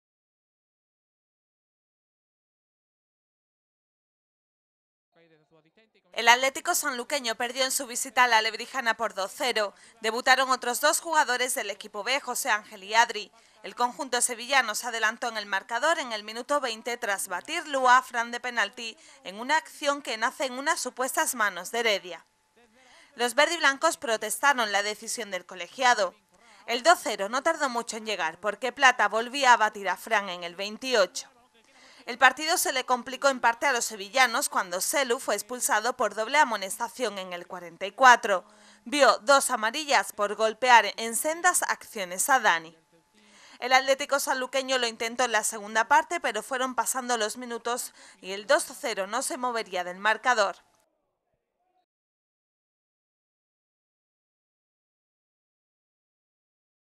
Noticias, Derrota del Atco. Sanluqueño en Lebrija
Esta vez no pudo ser.  El Atco. Sanluqueño no conseguía traerse los tres puntos de vuelta de Lebrija donde caía por dos goles a cero. Toca seguir y pensar en el próximo rival. Sanlúcar de Barrameda.